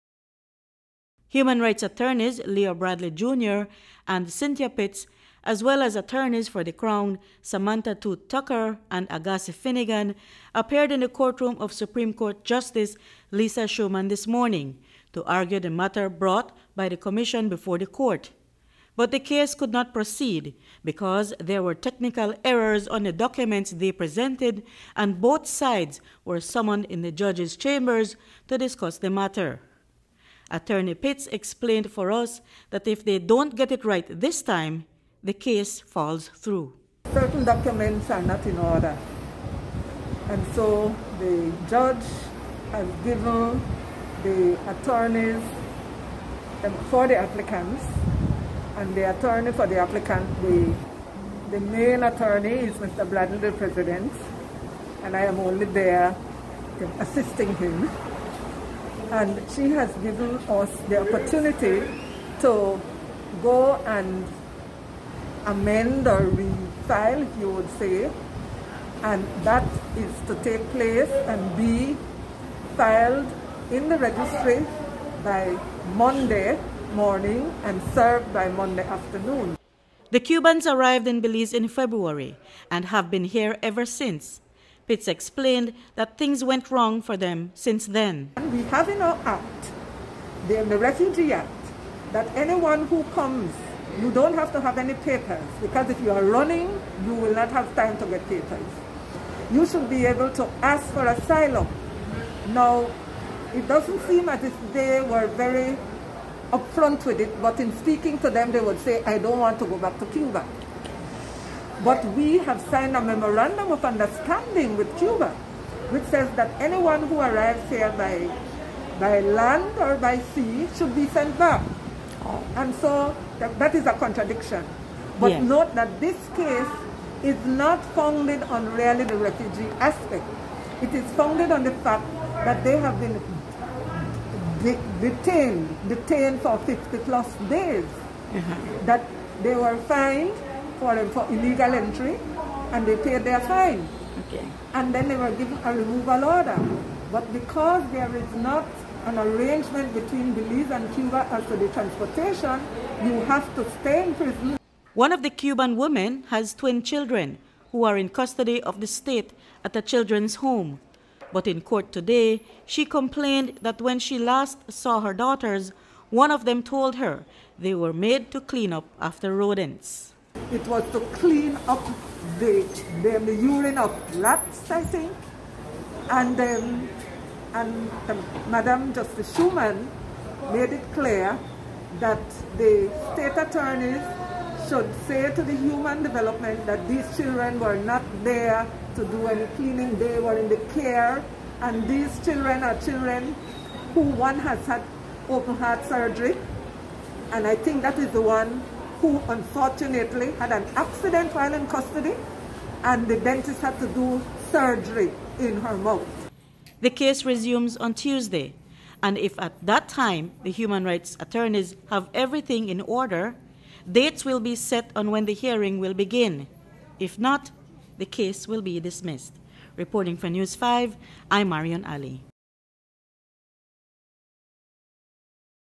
Judge Gives Human Rights Attorneys One Last Chance to Get it Right; Cubans Remanded Until Tuesday
The case of seven Cubans who are charged with immigration offences has been adjourned until next Tuesday. This is to give the Human Rights attorneys who have filed a case on their behalf to file proper affidavits and to serve the state by Monday afternoon. The Human Rights Commission had brought a suit against the state on grounds that the Cubans’ Constitutional rights had been violated because they were not informed in the language they understand what offence they had committed. But documents that the Commission attorneys have filed, including new ones filed this morning, contain errors. News Five’s Marion Ali was in the courtroom and filed the following report.